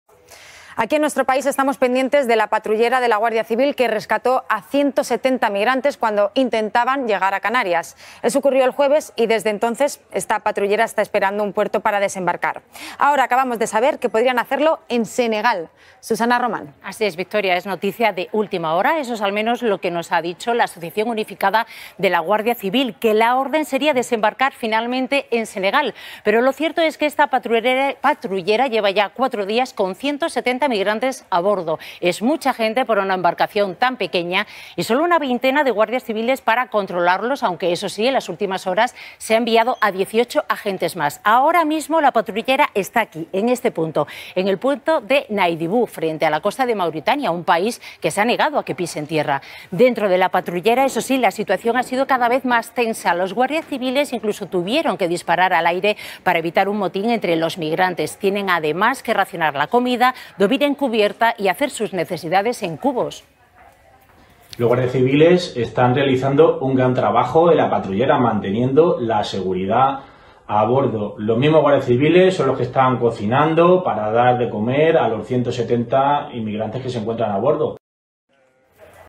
SENEGAL PODRÍA ACOGER A LA PATRULLERA ESPAÑOLA CON 170 MIGRANTES RESCATADOS POR LA GUARDIA CIVIL.
¿Quién ha dado esta orden? Solo puede partir de Ministerio de Interior o de Defensa Previo permiso del Presidente del Gobierno de España ¿En base a que derecho o legislación? . 30 Agosto 2023.
La patrullera se encuentra desde este jueves en alta mar a la espera de que sea acogida. Además, junto a los 170 migrantes también se encuentran 38 agentes de la Guardia Civil.
Desde el pasado jueves, una patrullera con 170 migrantes se encuentran en medio del mar a la espera de encontrar un puerto en el que poder desembarcar. En dicha patrullera también se encuentra una veintena de agentes de la Guardia Civil, quienes rescató a los migrantes cuando intentaban llegar a Canarias.
Sin embargo, parece que Senegal podría ofrecerse para ser el país que acoja la patrullera, según ha informado la Asociación Unificada de la Guardia Civil. Ahora mismo la patrullera se encuentra en el puerto de Nuadibú, frente a la costa de Mauritania, pero el país se niega a que pisen tierra.
Al parecer, la orden sería desembarcar finalmente en Senegal, pero la situación en la patrullera empieza a ser cada vez más tensa, ya que llevan 4 días a la deriva conviviendo un total de 208 personas, los 170 migrantes más 38 guardias civiles, 18 agentes más que se han enviado en las últimas horas.
En la patrullera se han vivido momentos de tensión e inquietud sobre qué podría pasar con ellos, ya que los agentes han tenido que disparar al aire para evitar un motín entre los migrantes. Además, la situación cada vez es más insostenible, pues muchos tienen que dormir en la cubierta y las condiciones higiénicas no son las debidas.